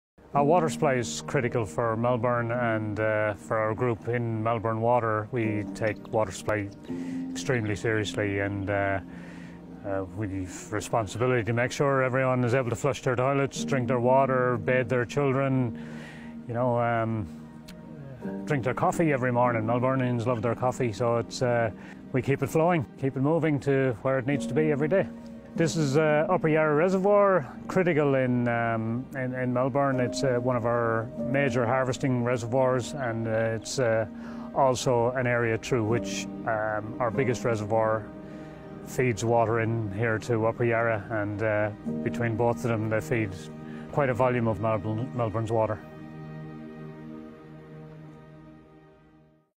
Upper Yarra Water Supply
Hey Melbourne, meet Tommie Conway. He's part of our Water Network Operations crew.
His team is responsible for moving water around our supply network to ensure we have the right water supply for Melbournians now and for the future.